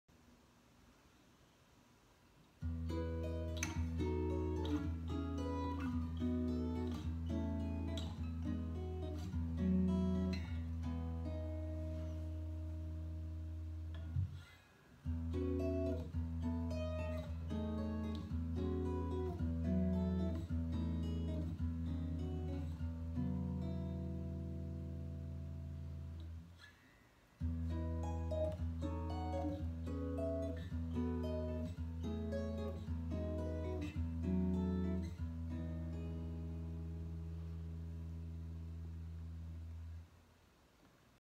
『Ted's Top Tips Descending Diatonic』ギタリスト市川強
Descending diatonic progression, using 3 different voicings, in the key of E.
3rd stacks, 4th chord, regular
キーEでダイアトニックコードだけで下がってます。
３度積み、４度積み、普通のボイシング(Drop2)